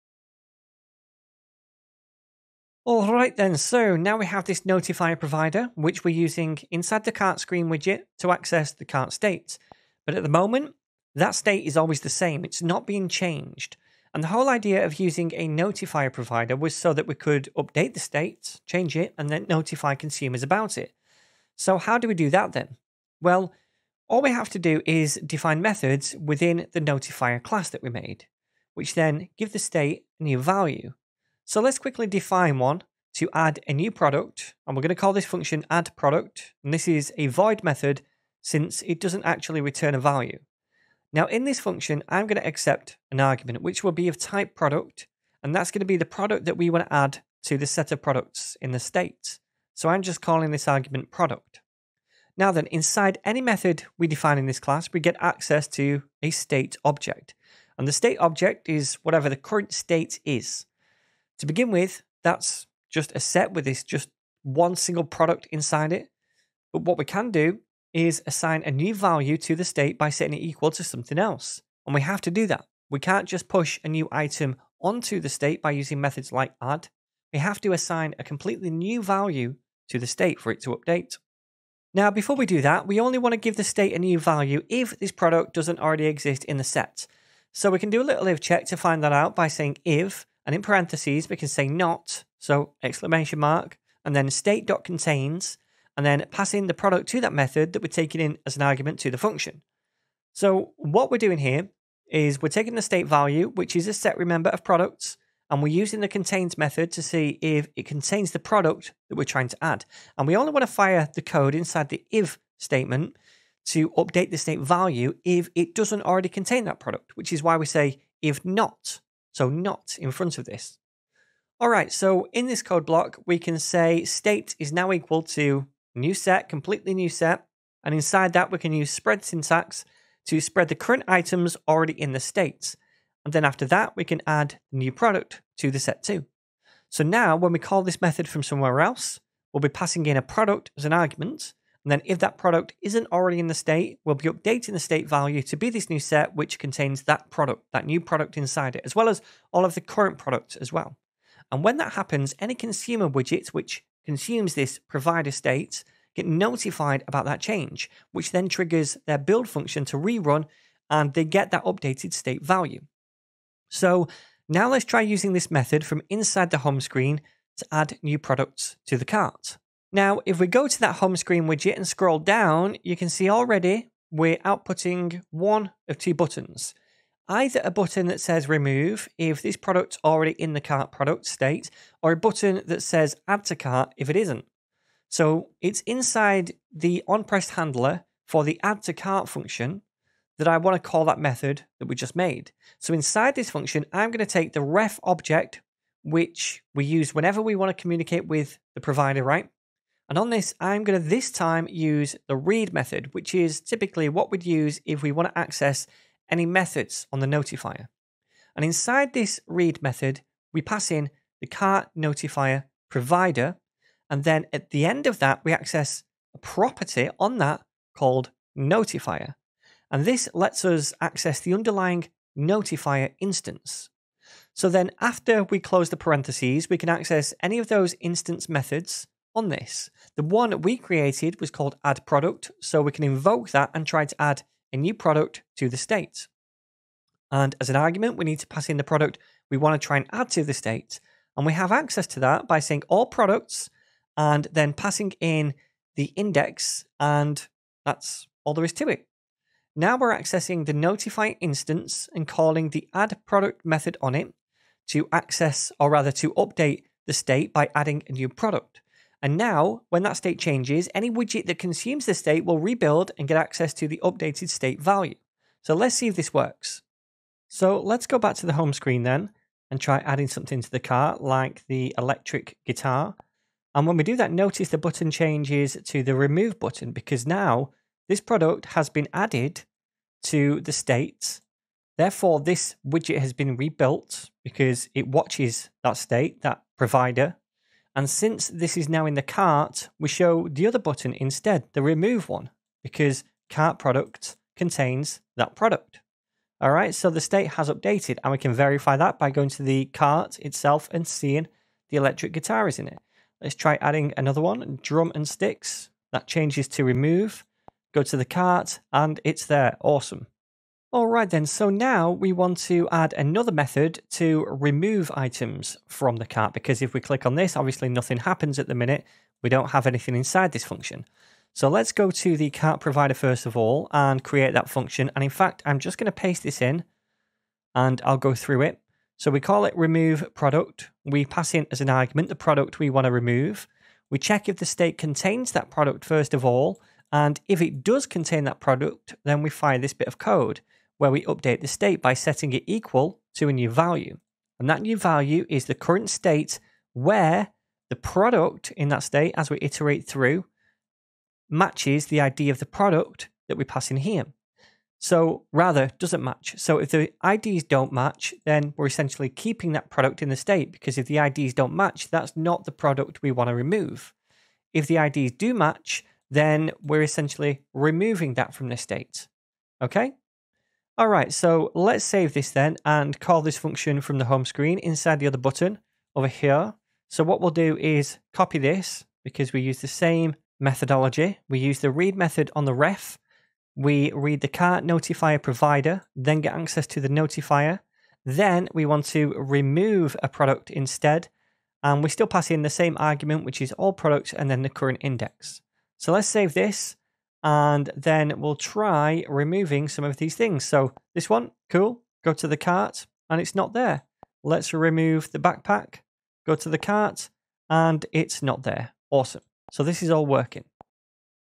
Riverpod Crash Course #8 - Updating State
In this Riverpod tutorial series, you'll learn how to use Riverpod providers to manage state in Flutter applications.